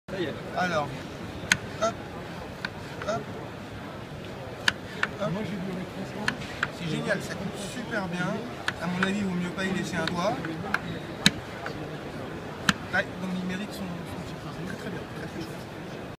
TEST - LA GUILLOTINE À SAUCISSON - SO APÉRO
Primée au salon Maison Et Objet Paris 2014, découvrez la guillotine à saucisson. C'est juste bluffant pour découper à une vitesse incroyable un saucisson !

N’hésitez pas à me dire en commentaire ce que vous pensez de cette recette de cuisine facile et rapide.

Si vous l’avez faite ?
Si vous comptez la tester ?
Si elle vous donne faim ?